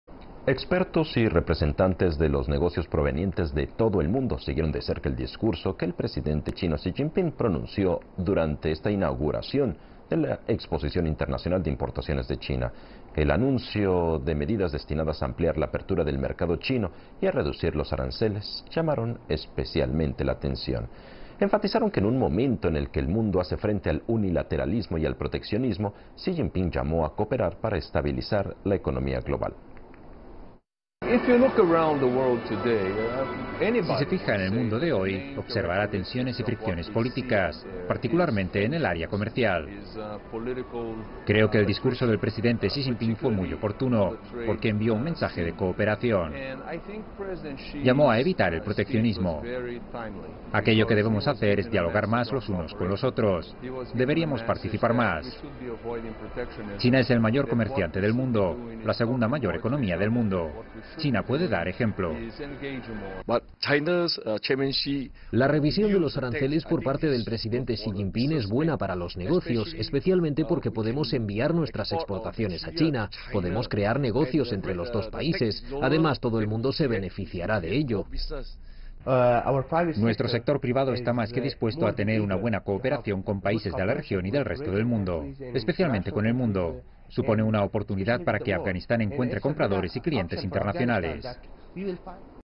Expertos internacionales ven oportunidades tras el discurso de Xi Jinping
Expertos y representantes de los negocios provenientes de todo el mundo siguieron de cerca el discurso que el presidente chino, Xi Jinping, pronunció durante la inauguración de la Exposición Internacional de Importaciones de China. El anuncio de medidas destinadas a ampliar la apertura del mercado chino y a reducir los aranceles llamaron especialmente la atención. Asimismo, enfatizaron que, en un momento en el que el mundo hace frente al unilateralismo y al proteccionismo, Xi Jinping llamó a cooperar para estabilizar la economía global.